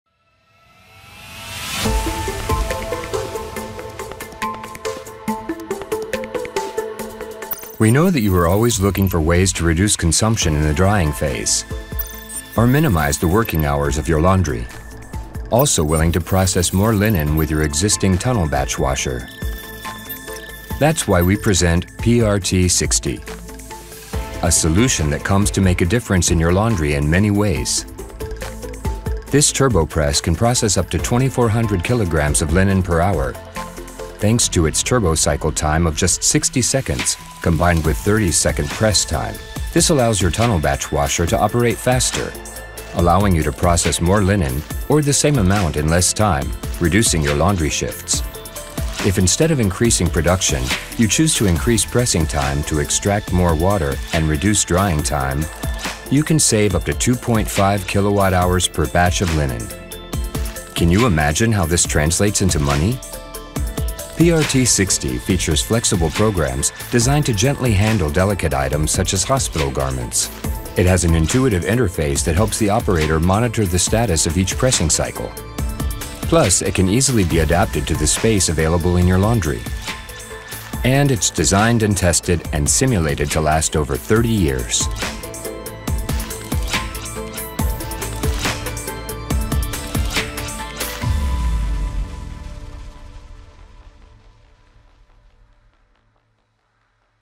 PRT60 - Girbau's newest turbo press for water extraction
Introducing the PRT60 Turbo Press by Girbau!
This cutting-edge press is designed to meet the demands of modern industrial laundries. With a 60-second turbo cycle time combined with a suitable press time, the PRT60 boosts productivity and cuts costs.
Ready to revolutionize your laundry process?